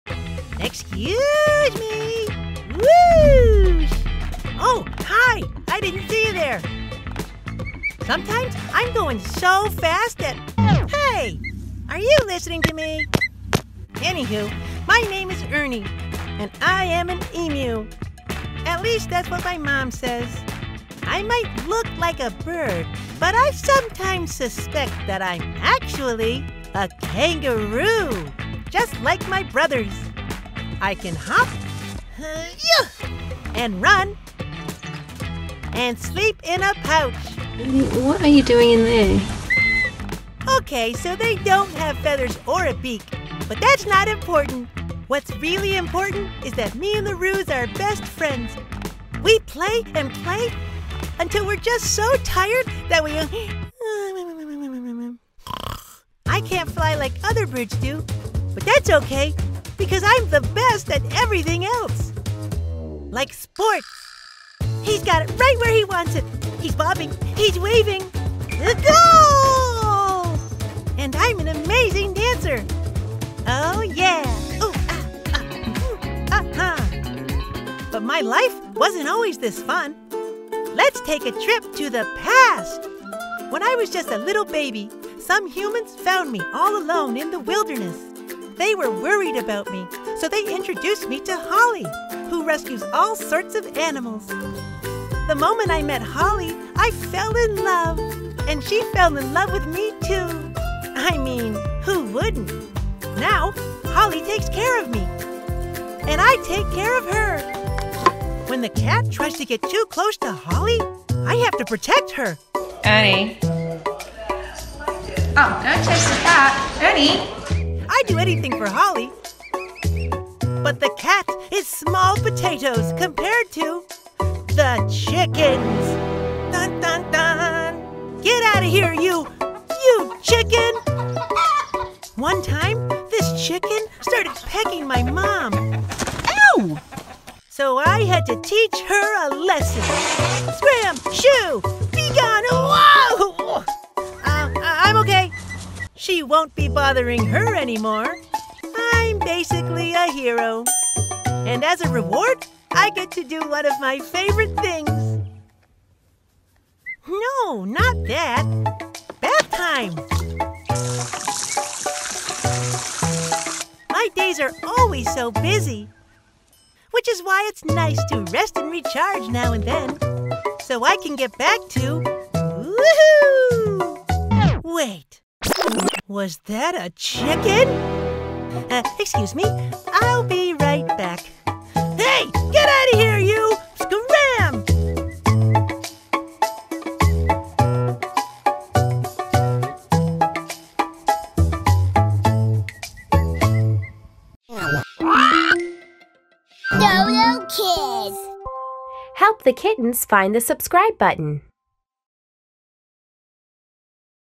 Ernie The Dancing Emu Has The Hoppiest Best Friends | It's Me! | Dodo Kids
Ernie the Emu loves his kangaroo brothers, his mom Holly, doing dance moves, running all over the place, taking baths — he loves LOTS of stuff. There's only one thing he doesn't love: CHICKENS. If you're a chicken, you better watch out!